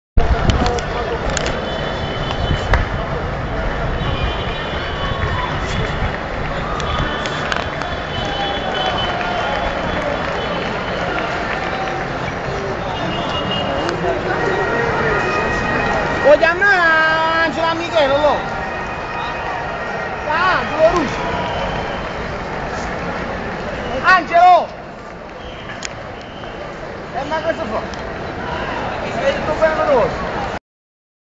autobus ssc napoli fuori al san paolo
arrivo dell'autobus dei calciatori della ssc napoli al san paolo contro la sampadoria